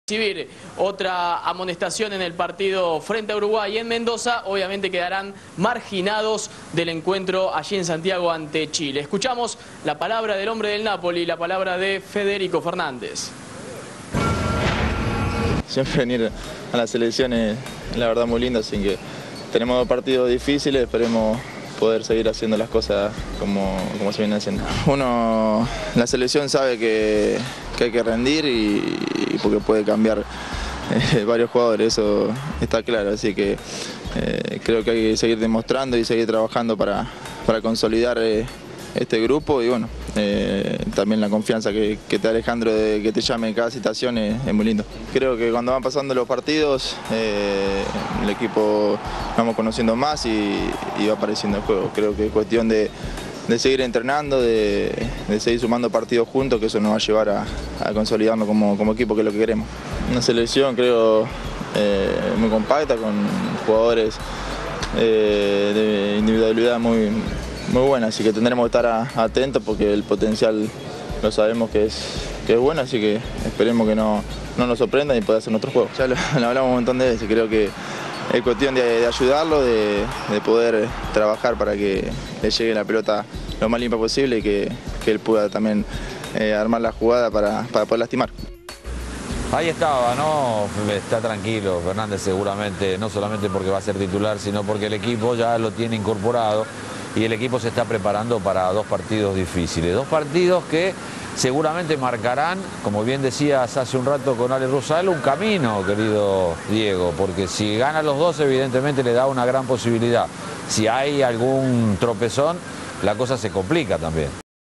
Federico Fernández su ESPN Argentina
Prima della gara contro l'Uruguay, venerdì prossimo, a Mendoza.